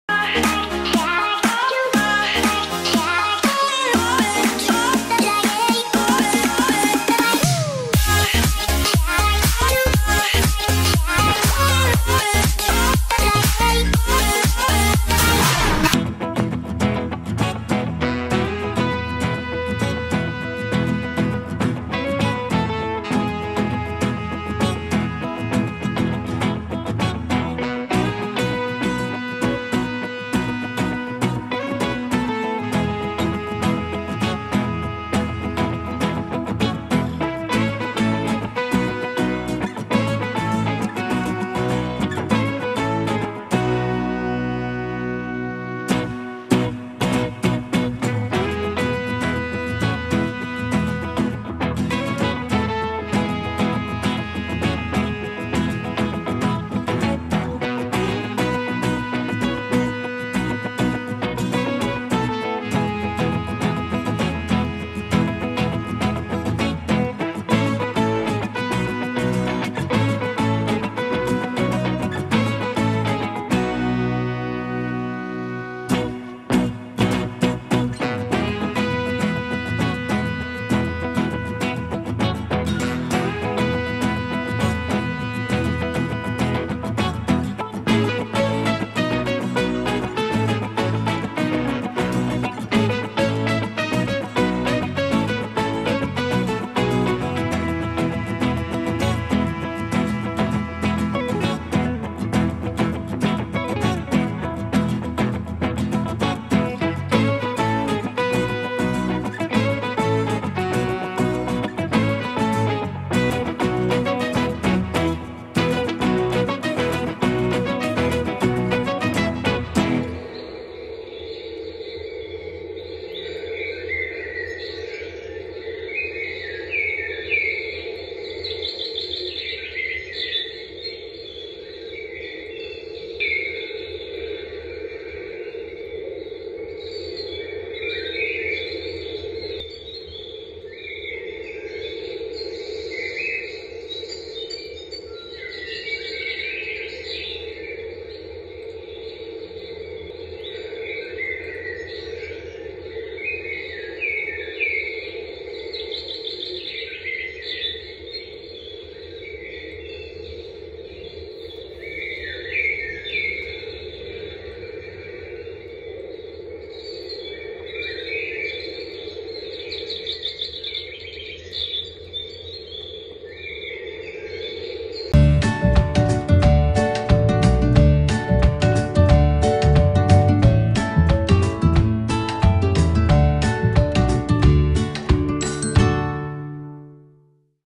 UNBOXING | YOUNG LIVING FEATHER THE OWL | Rhia de Guzman
My kids love Feather the Owl because it is so cute and has different sounds that makes them feel so relax using some of  their favorite essential oils.